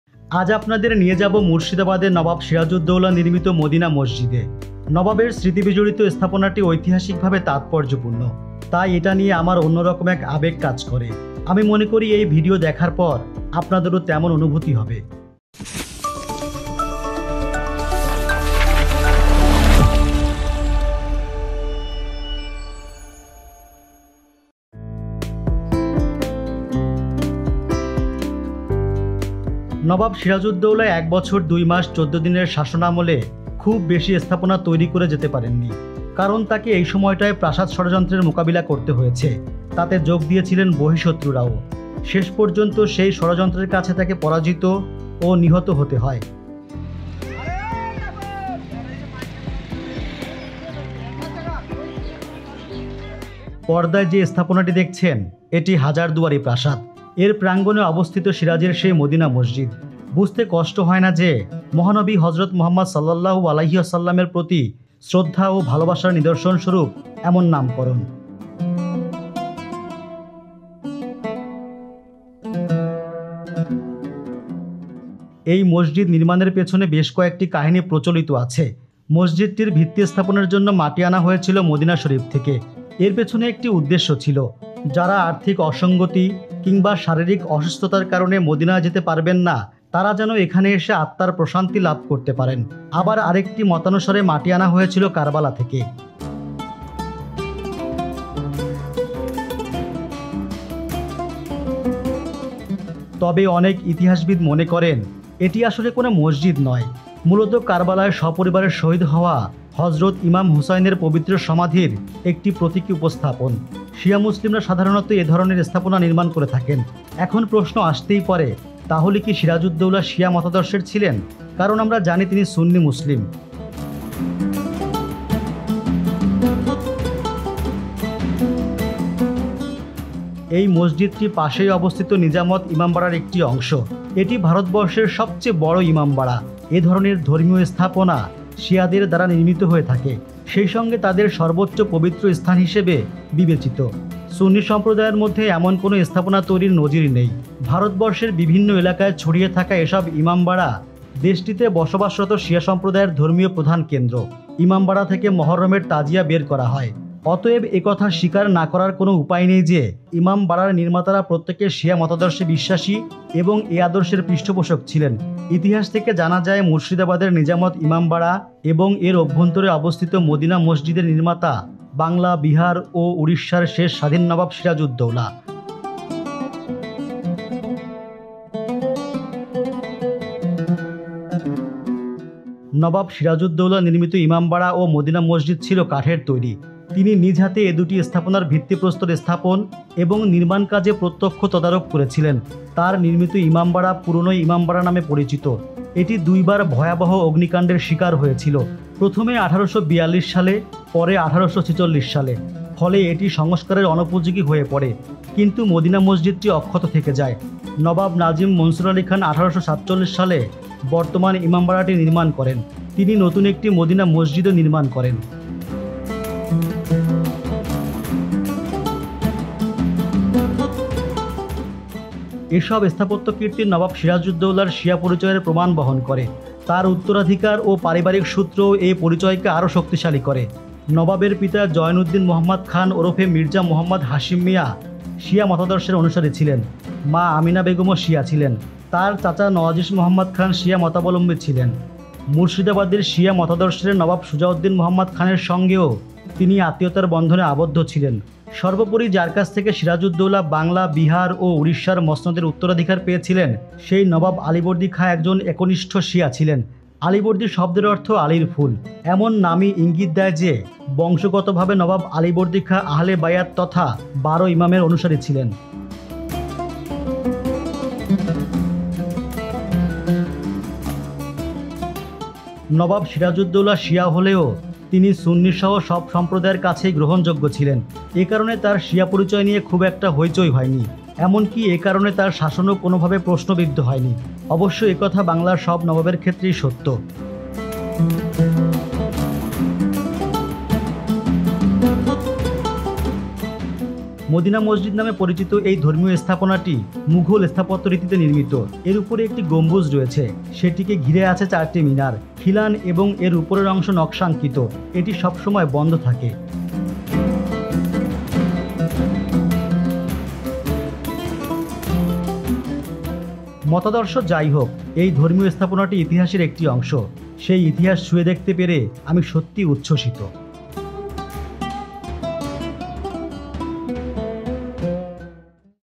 নবাব সিরাজউদ্দৌলার মদিনা মসজিদ | Madina Mosque | Rezwan Siddique
এই মসজিদ নির্মাণের পেছনে বেশ কয়েকটি কাহিনি প্রচলিত আছে। এর ভিত্তি স্থাপনের জন্য মাটি আনা হয়েছিল মদিনা শরিফ থেকে। এর পেছনে একটি উদ্দেশ্য ছিল। যারা আর্থিক অসংগতি কিংবা শারীরিক অসুস্থতার কারণে মদিনায় যেতে পারবেন না, তারা যেন এখানে এসে আত্মার প্রশান্তি লাভ করতে পারেন। আবার আরেকটি মত অনুসারে মাটি আনা হয়েছিল কারবালা থেকে।

There are several stories behind the construction of this mosque. Soil for its foundation was brought from Medina Sharif. There was a purpose behind it. Those who cannot go to Madinah due to financial inconsistencies or physical ailments, may come here and find peace of mind. According to another opinion, the soil was brought from Karbala.

This content is Copyright to Rezwan Siddique. Legal action will be taken against those who violate the copyright of the following material presented.